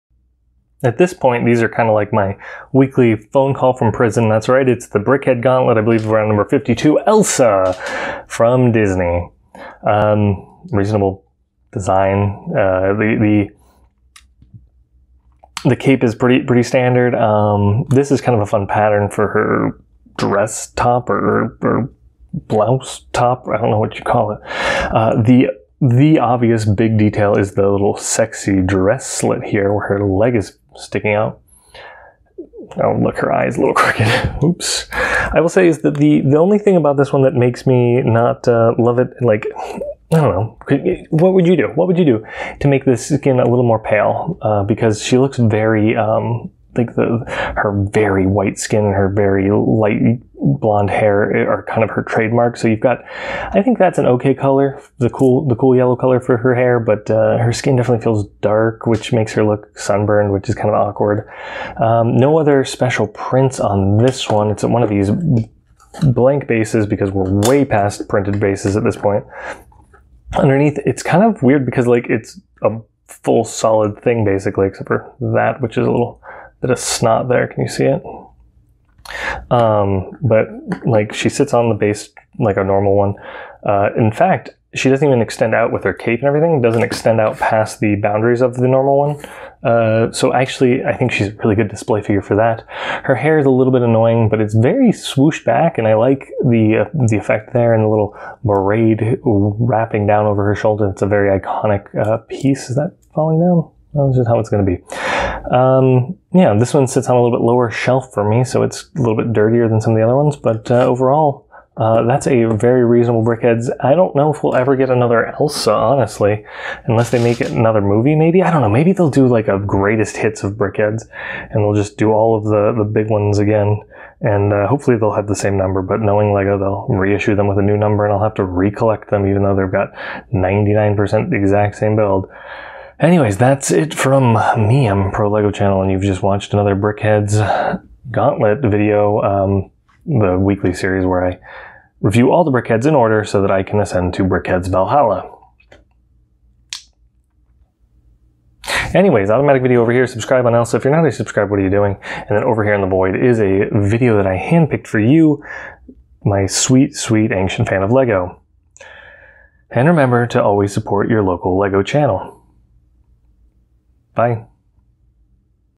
41617 Else Brickheadz Review (Disney Frozen 2018)
She's cool. I guess. Number 52 is down and we only have like 100 more as I publish this video.